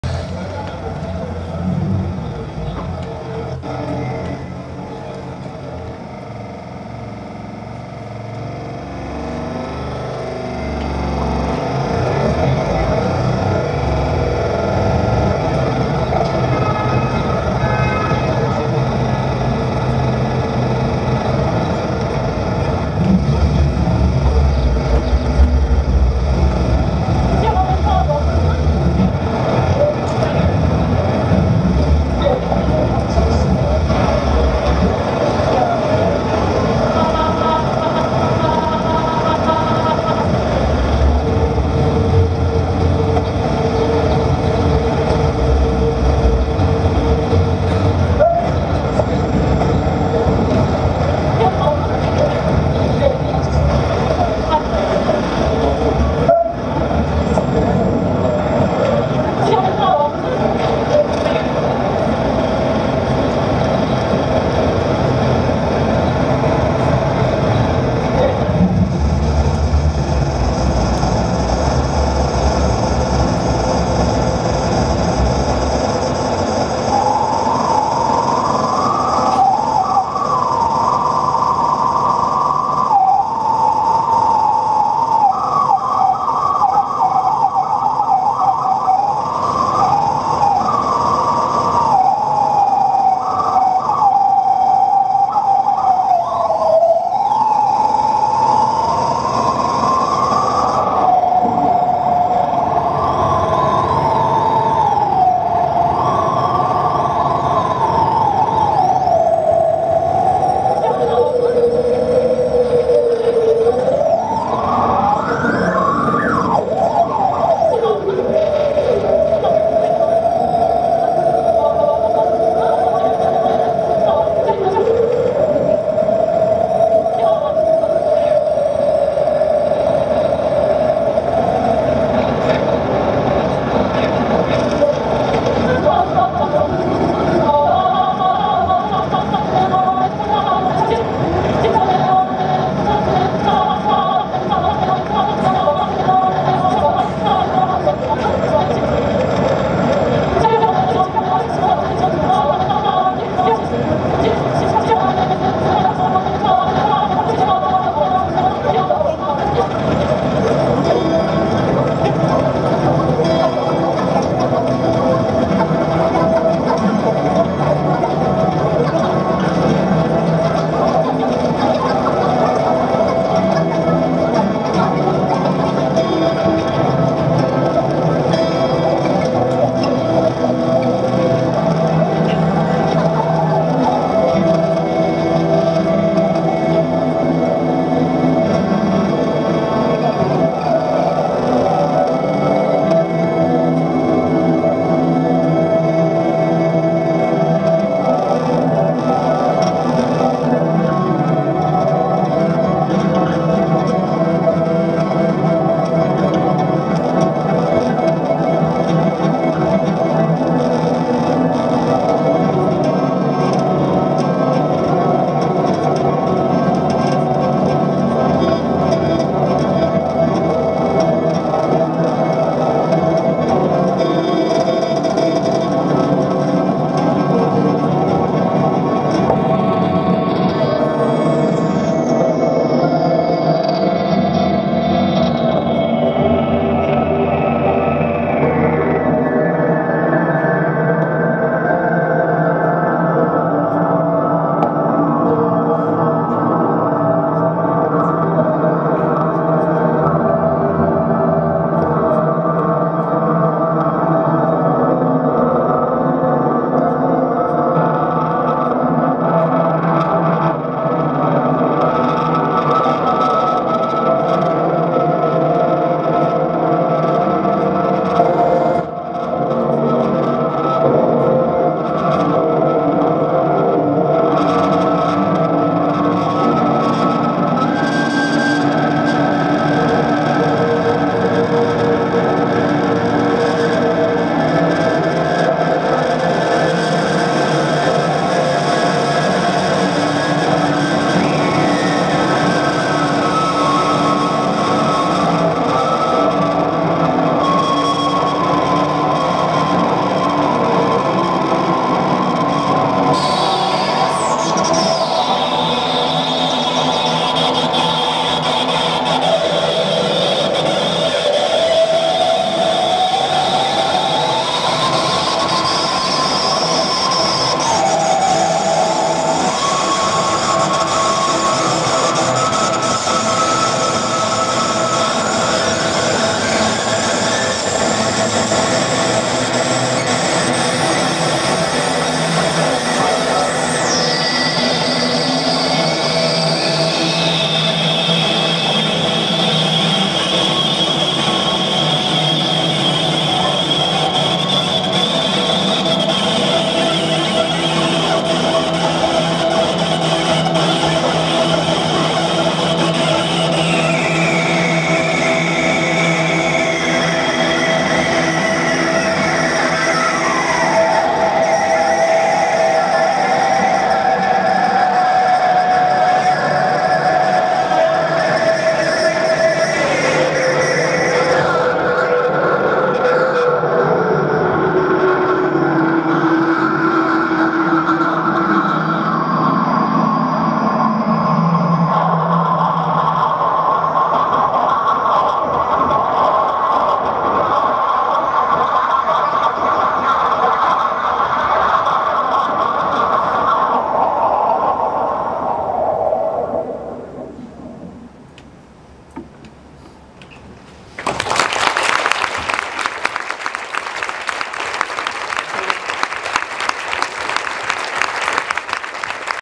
Atau Tanaka performs at his inaugural lecture
Atau Tanaka gave his inaugural lecture as part of the Made in Goldsmiths series, titled 'Getting Physical: Visceral Music Machines'.

He's joined Goldsmiths in the Department of Computing as Professor of Media Computing at Goldsmiths and CTO of Clicmobile. Atau Tanaka was born in Tokyo, and was raised in the U.S. He bridges the fields of media art, experimental music, and research.

Here's his performance which he gave after the lecture using two iPhones.